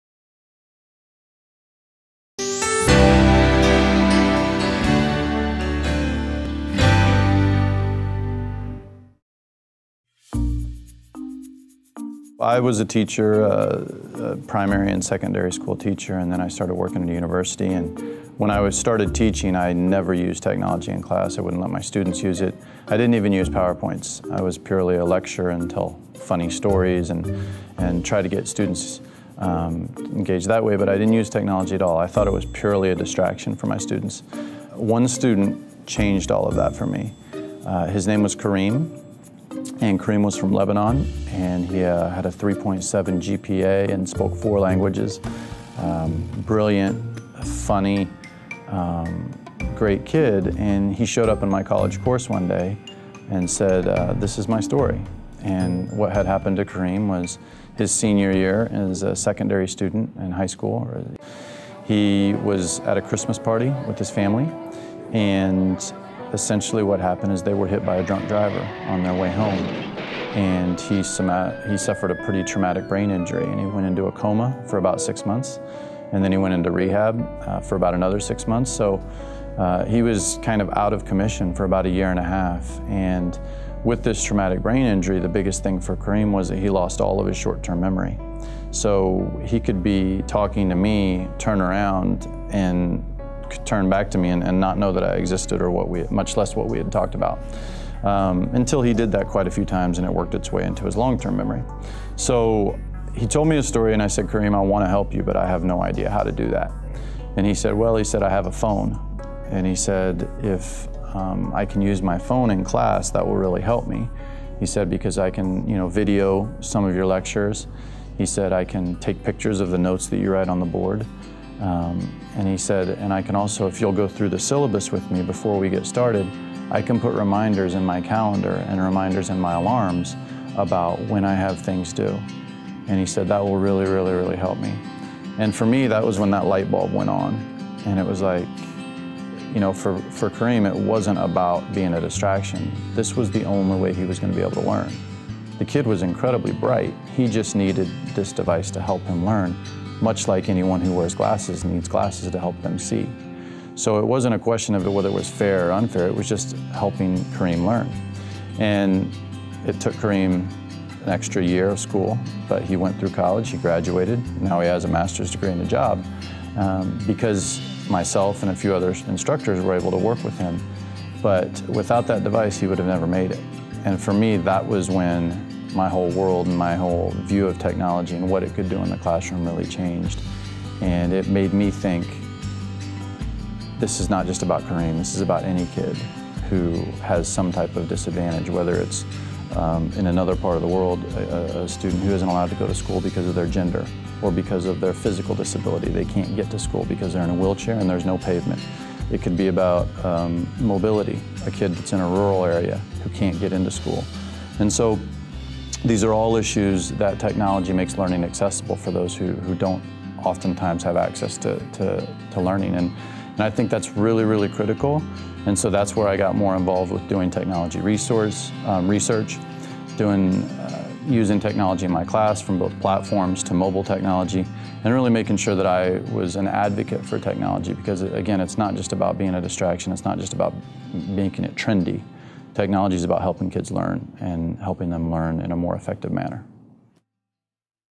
My Story
This is my story of how I became involved with technology.  What is your story?